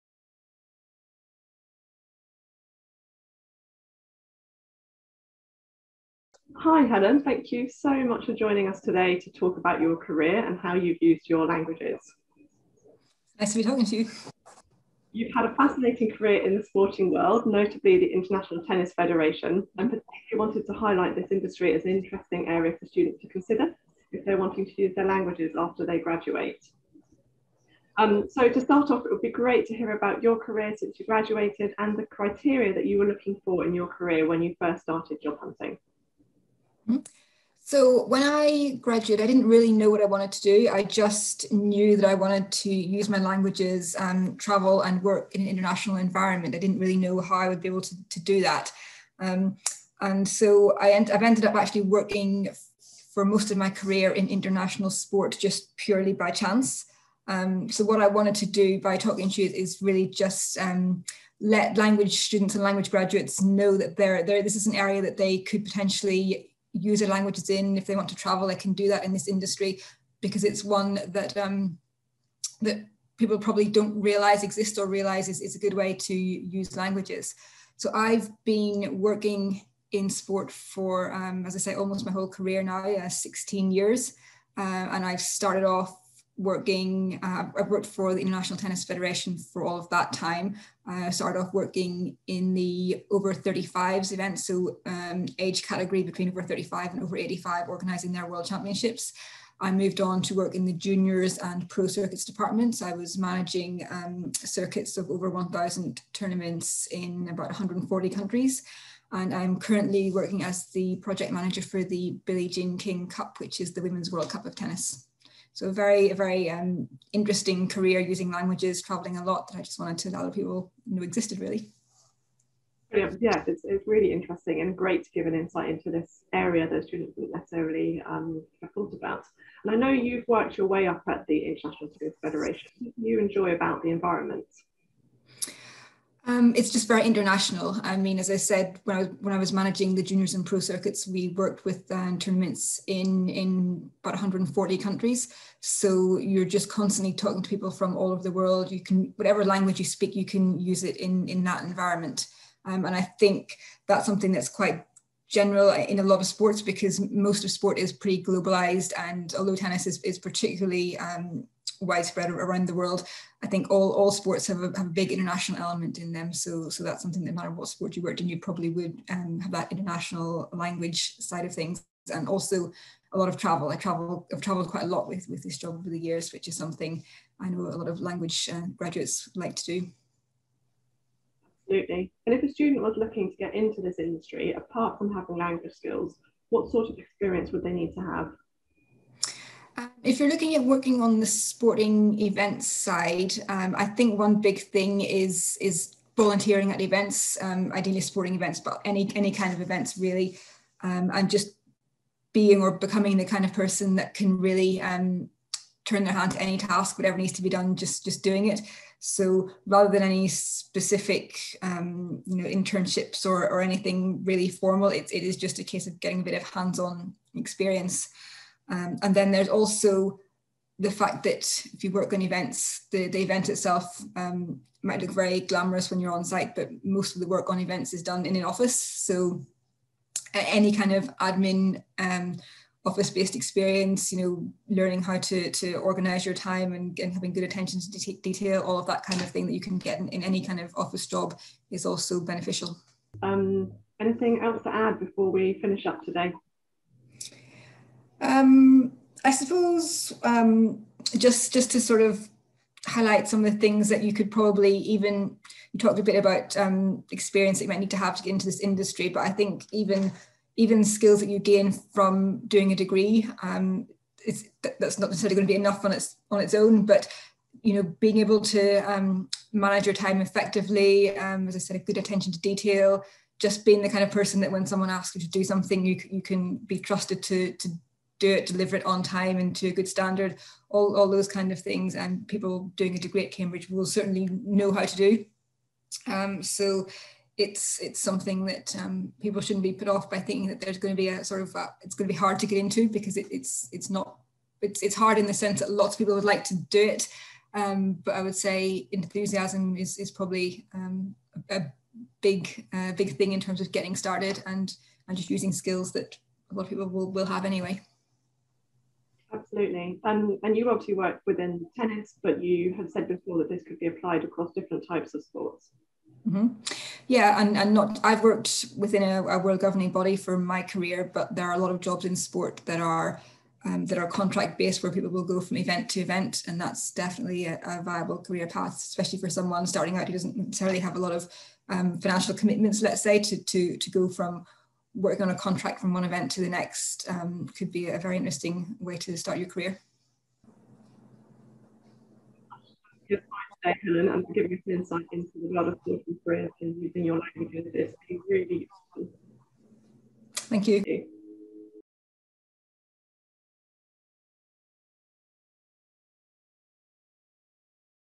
Careers using Languages - Helen McFetridge - sporting world
Helen works for the International Tennis Federation and has always had lots of use for her language skills in her roles here. In this video she talks about this sector as a way to use your languages, and talks about related areas and how to get experience.